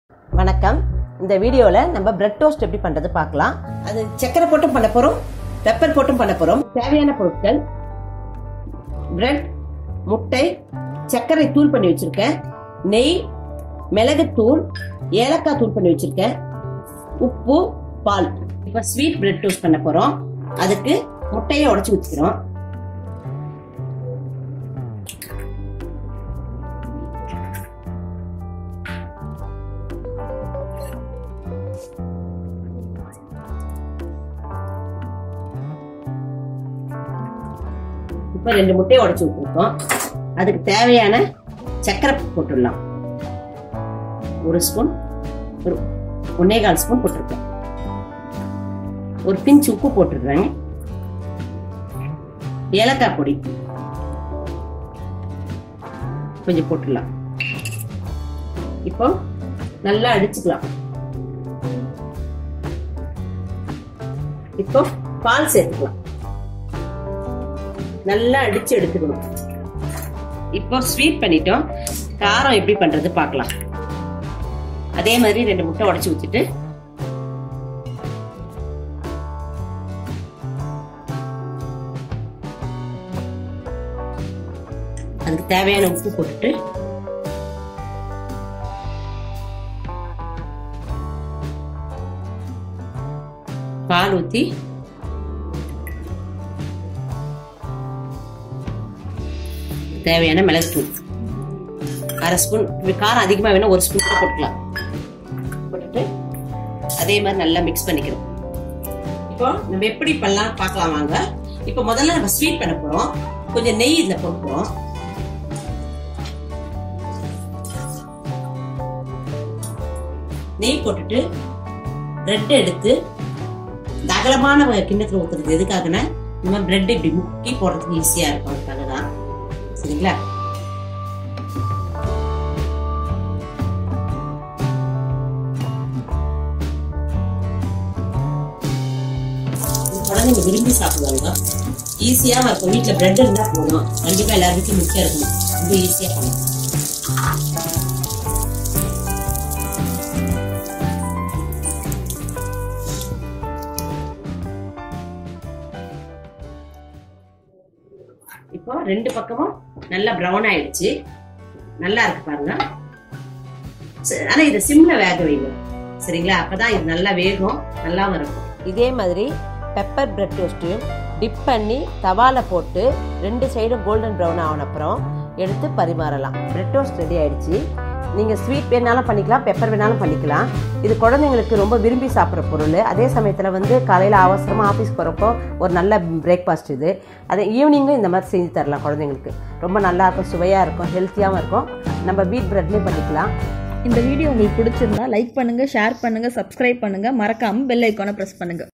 பிரட் டோஸ்ட் செய்முறை | Bread Toast in Tamil | Sweet & Spicy | French Toast
Hello, In this video I have prepared Egg Bread Toast in both sweet and spicy flavour.  Try this at home and give your feedback in the comment section.

உங்கள் வீட்டில் இதை முயற்சி செய்து உங்கள் கருத்தை கூறவும்